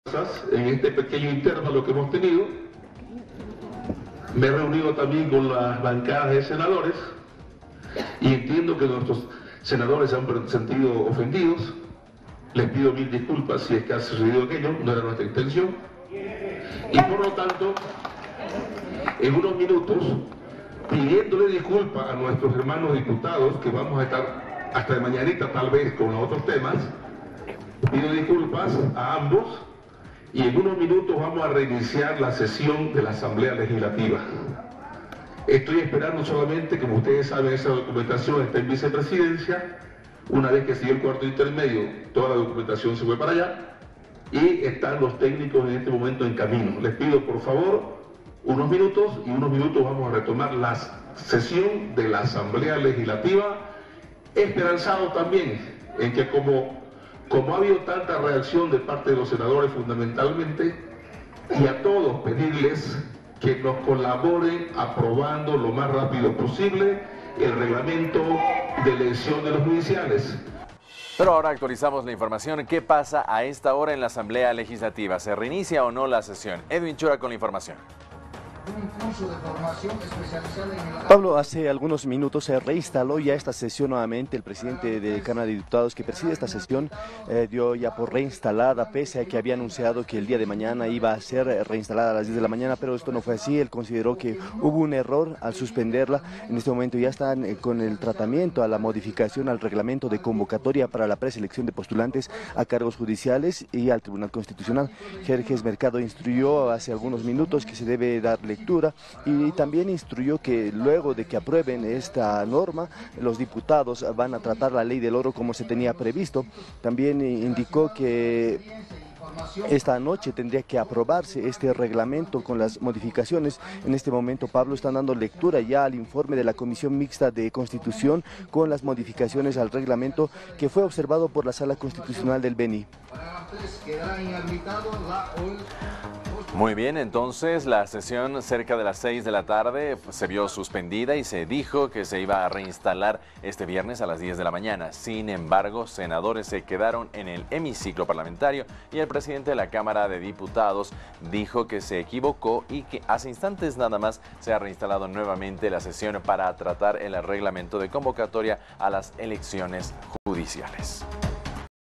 Asamblea reinstala sesión para tratar el reglamento de selección de candidatos judiciales
Tras haberse anunciado este jueves un cuarto intermedio hasta el viernes en la Asamblea Legislativa Plurinacional (ALP) para abordar el reglamento de preselección de candidatos a elecciones judiciales, cerca de las 19:00 el anunció quedo en nada, pues se decidió reinstalar la sesión como en principio se había anunciado.